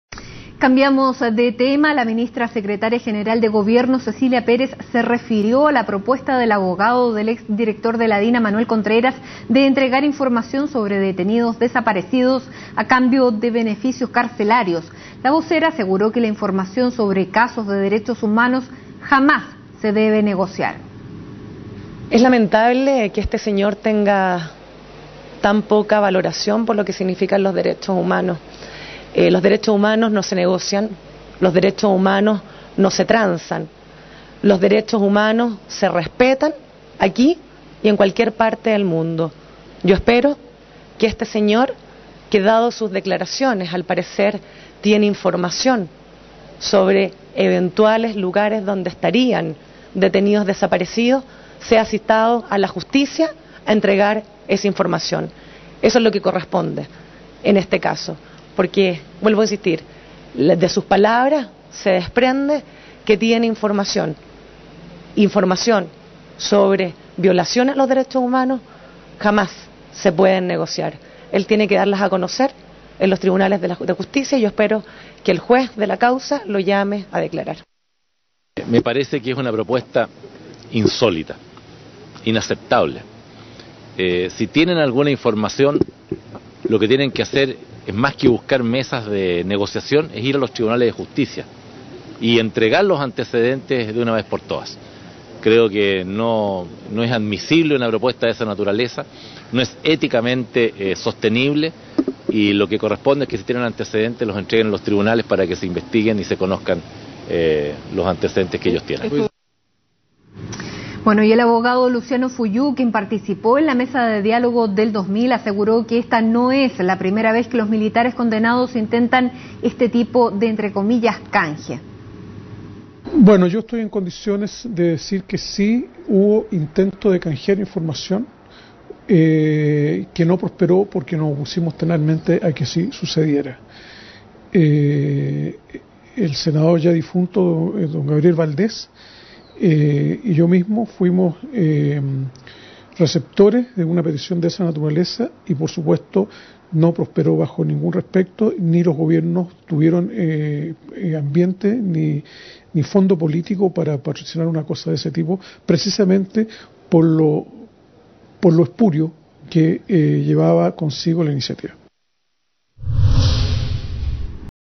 Desde el oficialismo descartaron "negociar" con violadores de Derechos Humanos
El abogado de Manuel Contreras realizó un llamado para entregar información.